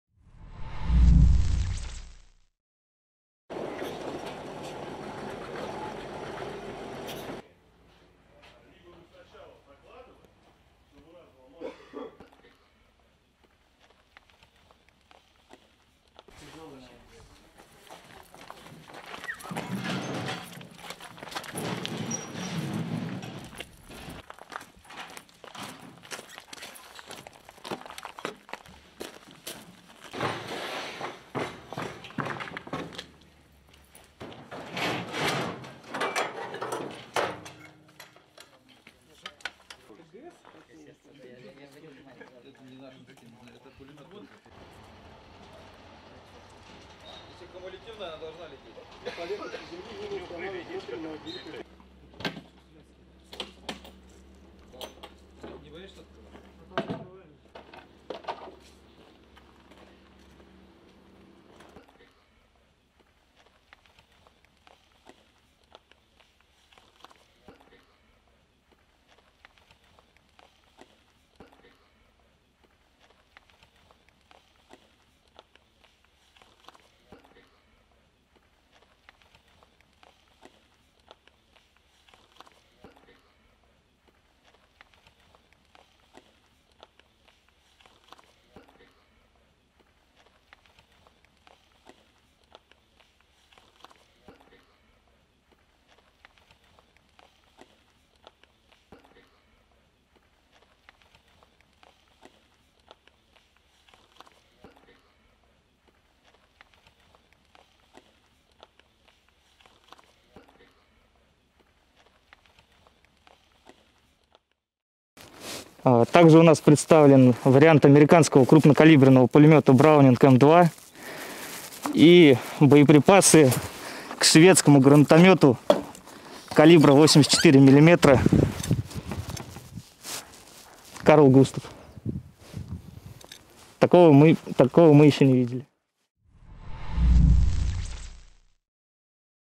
Russia Flaunts Stockpile Of Captured Western Arms Abandoned By Ukrainian Troops
This is the moment a Russian soldier shows off the stockpile of captured Western weapons allegedly abandoned by fleeing Ukrainian troops.

The Russian military said the captured weapons included Javelin rocket launchers, Browning M2 heavy machine guns, and Carl Gustaf grenade launchers.

The Russian Ministry of Defence (MoD) said on 13th June: "Captured weapons hastily abandoned by the retreating units of the Armed Forces of Ukraine."

The Russian MoD added: "Russian military personnel note that soldiers of the Armed Forces of Ukraine began to increasingly abandon their American 'Javelin' anti-tank missile systems and Swedish-produced NLAW [Next generation Light Anti-tank Weapon] imported by Western countries due to their bulkiness."

In the footage, Maxim, the head of the depot where the captured weapons are being stored, shows the stockpile of arms and tells the camera: "Here, we also have a version of the American Browning M2 heavy machine gun and ammunition for the Swedish 84-mm Carl Gustaf grenade launcher."

He adds: "We have not seen anything like this before."

We contacted Russian and Ukrainian officials for comment but had not received a reply at the time of writing.

Russian troops invaded Ukraine on 24th February in what the Kremlin is still calling a "special military operation". Today marks the 110th day of the campaign.

The General Staff of the Armed Forces of Ukraine reported that between 24th February and 12th June, Russia had lost 32,150 personnel, 1,430 tanks, 3,484 armoured combat vehicles, 715 artillery systems, 226 multiple launch rocket systems, 97 air defence systems, 212 warplanes, 178 combat helicopters, 582 drones, 13 boats, 2,455 motor vehicles and fuel tankers, 54 pieces of special equipment and 125 cruise missiles.

A huge fire is reportedly raging at a chemical plant in Sievierodonetsk as street-to-street fighting between Ukrainian and Russian forces takes place in the city.

Ukrainian President Volodymyr Zelenskyy has said that Russian attacks on the cities of Sievierodonetsk and Lysychansk has turned them into "dead cities", but added that Ukrainian forces could still hold Sievierodonetsk despite being outnumbered.

An aide to President Zelenskyy has said that Ukraine is losing up to 200 troops a day.

President Zelenskyy has asked for additional weapons from Ukraine's allies to help push back the Russian forces.

The United Kingdom and the United States are sending long-range missile systems to Ukraine. Russian President Vladimir Putin has warned of more attacks in Ukraine if the West sends long-range weapons.